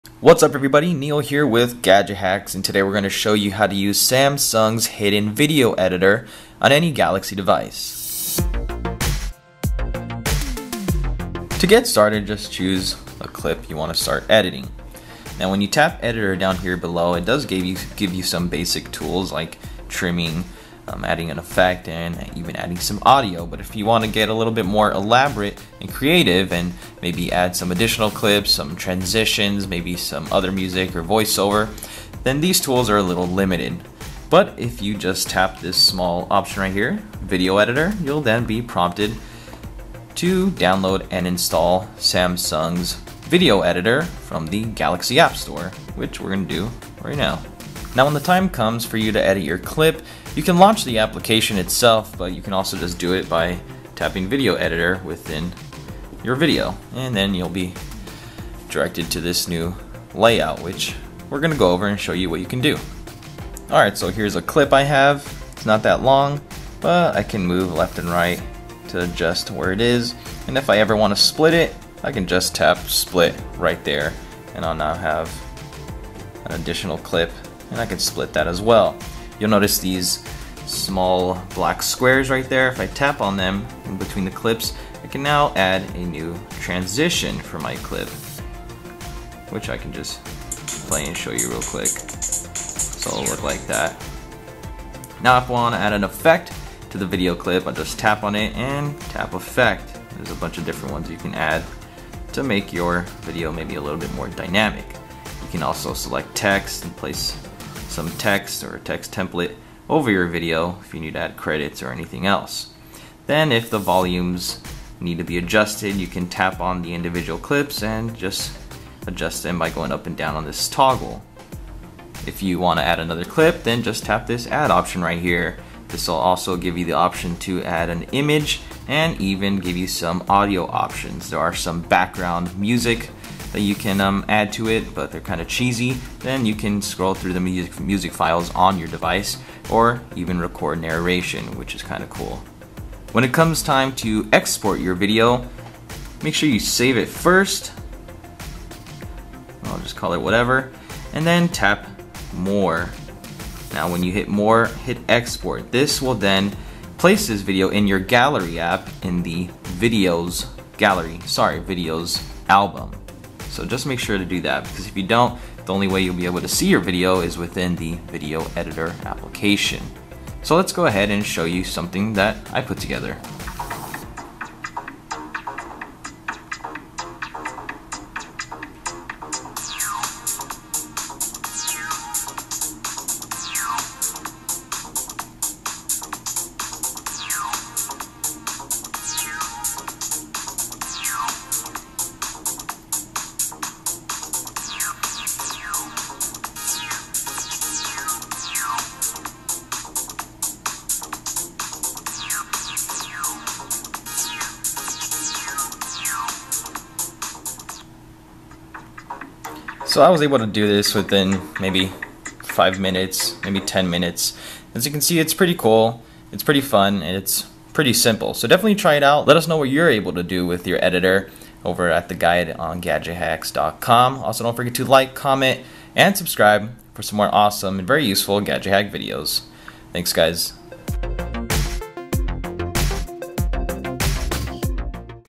Use Samsung's Hidden Video Editor on Any Galaxy Device [How-To]
How to Use Samsung's Hidden Video Editor on Any Galaxy Device

In this video, I'll be showing how to use the hidden video editor on your Galaxy device. With it, you can splice and split videos, add music or voiceovers, and place a text overlay. With more useful tools like transitions and voiceover audio, this editor will help make your videos much more dynamic and entertaining.